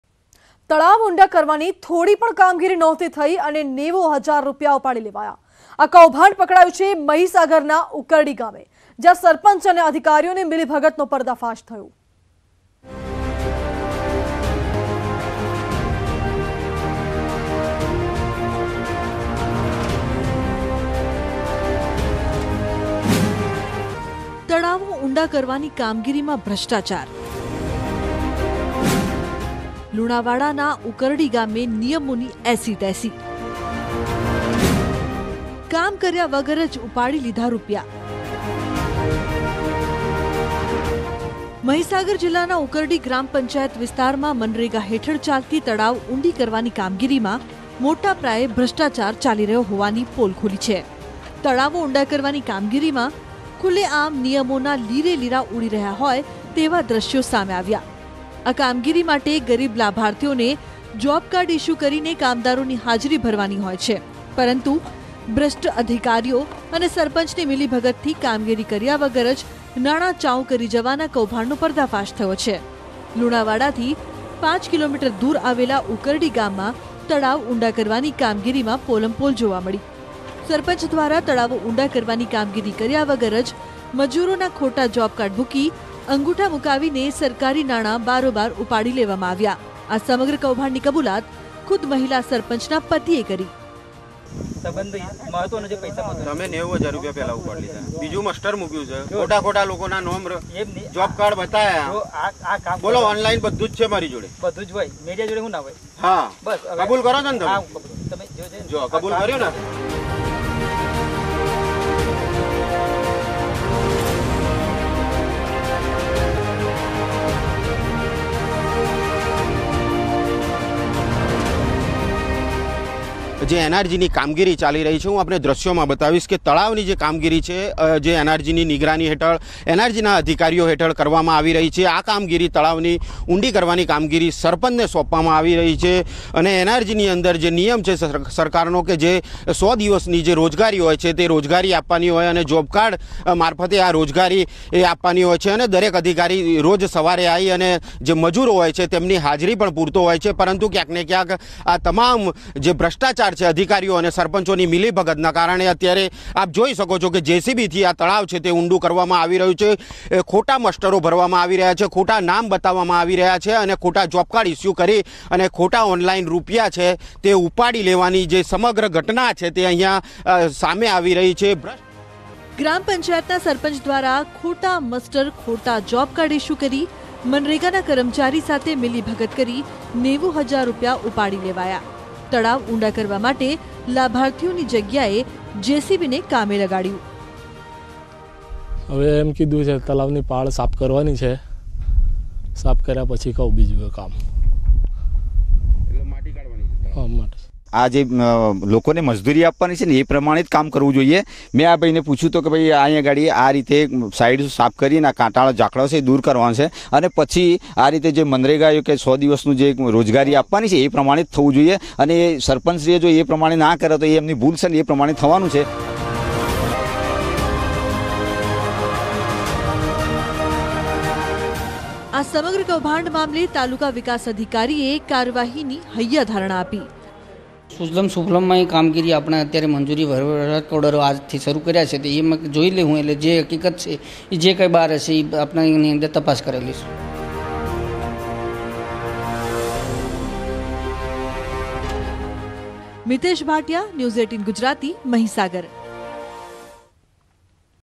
Mahisagar : તળાવો ઊંડા કરવાની કામગીરીમાં ભ્રષ્ટાચાર | Corruption | News18 Gujarati
Mahisagar ના લુણાવાડાના ઉકરડી ગામમાં તળાવો ઊંડા કરવાની કામગીરીમાં ભ્રષ્ટાચાર થયો.  તળાવ ઊંડા કરવાની કામગીરીમાં ખુલ્લે આમ નિયમોના લીરે લીરા ઉડ્યા.ભ્રષ્ટ અધિકારીઓ અને સરપંચની મિલીભગતથી કામ કર્યા વગર જ નાણા ચાઉં કરી જવાનું કૌભાંડ સામે આવ્યું.